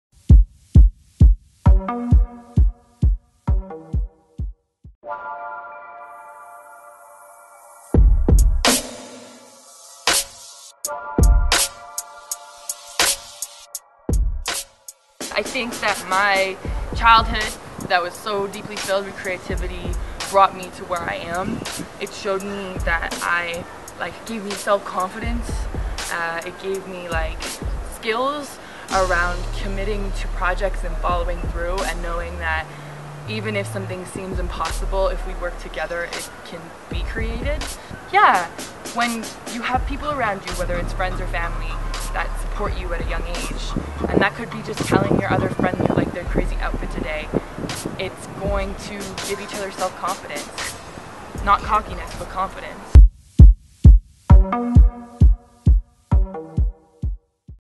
Cydney Eva on support from her community
Travelling Couch Arts Experience discuss all things art with one of the  a creative young Artist Cydney Eva, who is part of Pattern Nation @pattern.nation , @cydeva on instagram.